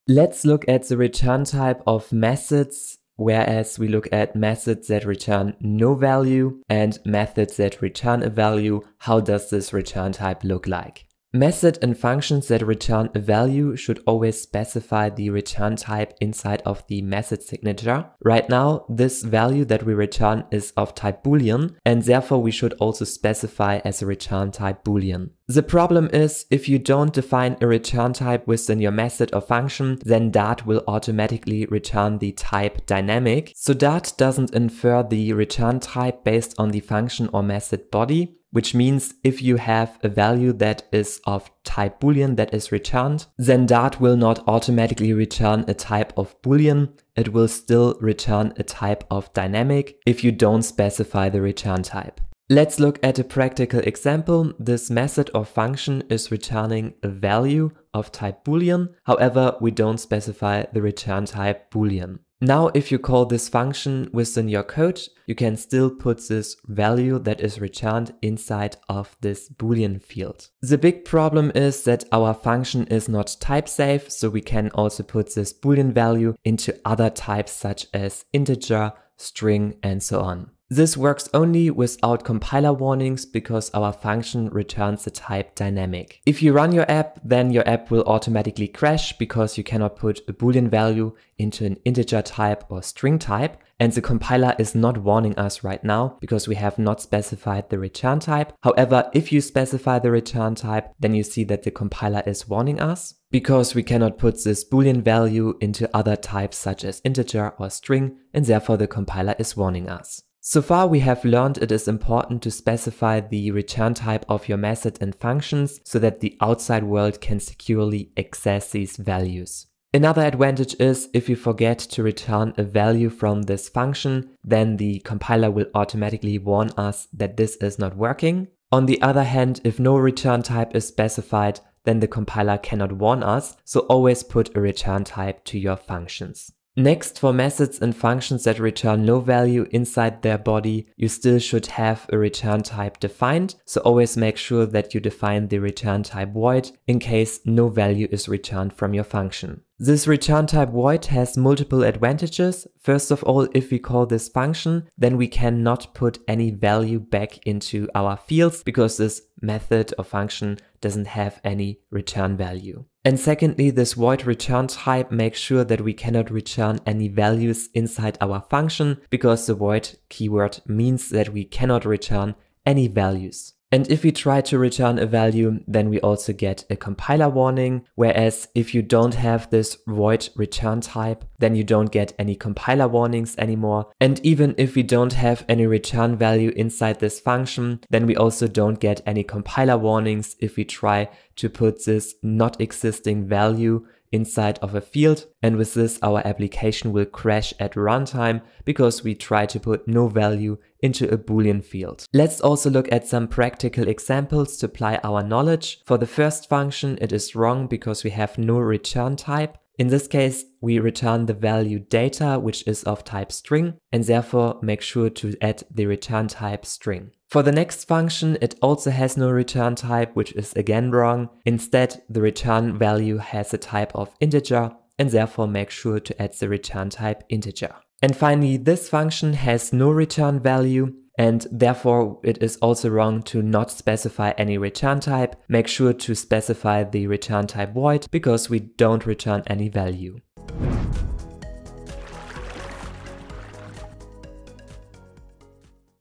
Write Clean Flutter Code - Return Type
How to write high quality and clean Flutter code. For this you need to know how to define return types correctly in Dart and Flutter.